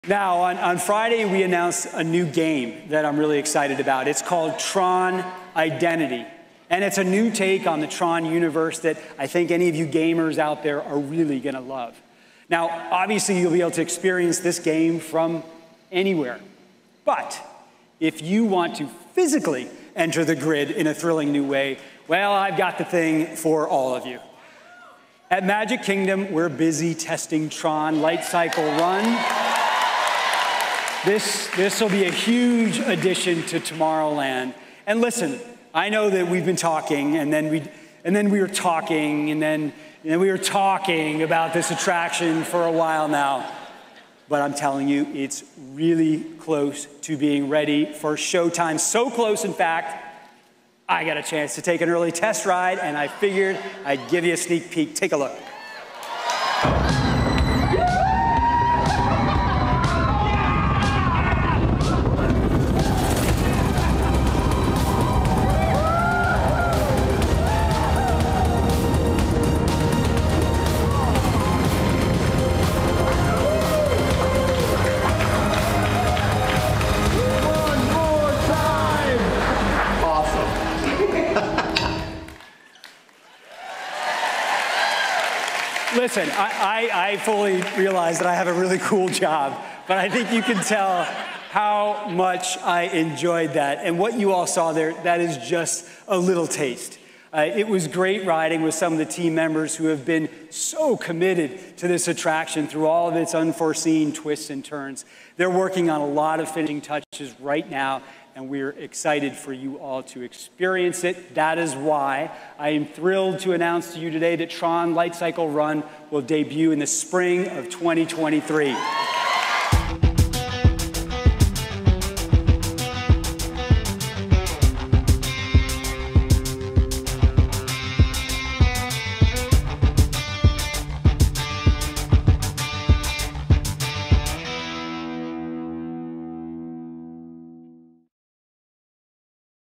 TRON Lightcycle / Run FINALLY Opening In Spring 2023 - Disney D23 Expo Confirmation
D23 Expo: The Ultimate Disney Fan Event returns to the Anaheim Convention Center from Sept. 9-11, 2022, and its full schedule of panel discussions has just been released